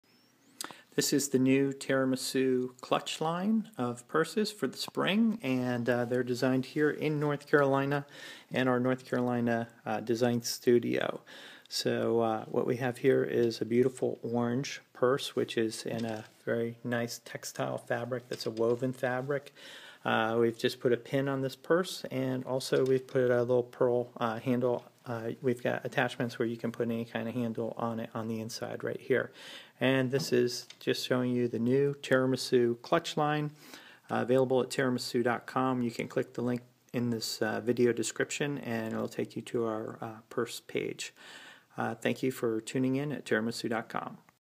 Teramasu Orange Clutch Purse
Elegant Teramasu Orange Clutch Purse with designer Black and White trim.